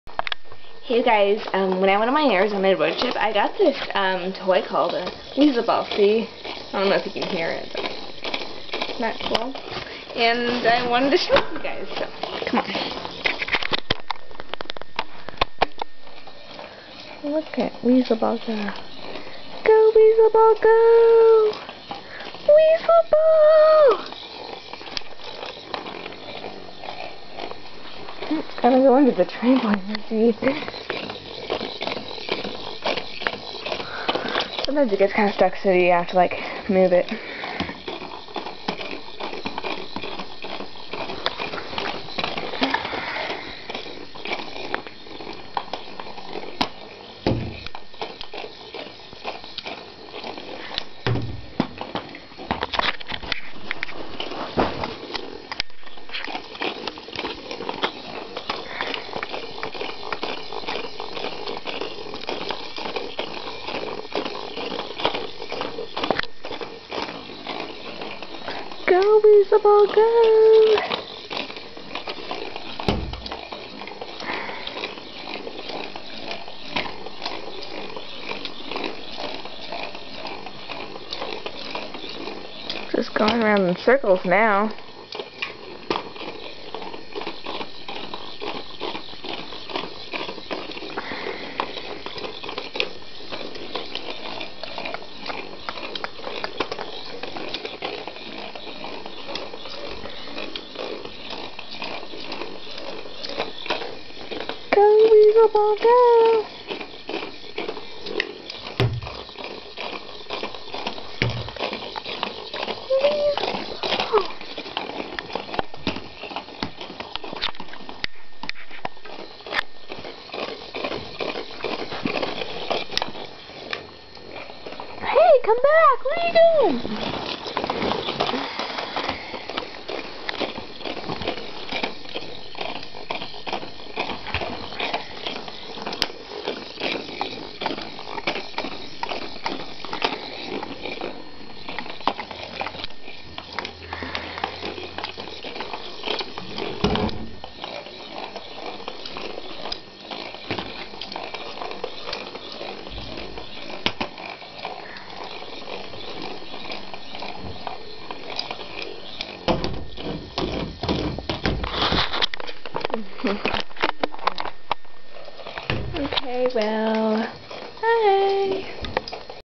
Weasel Ball!
Whaile I was on my Arizona Road Trip, we stopped at this restrant that had a oy store in it and, I decided to get my first Weasel Ball. Go Weasel Ball go!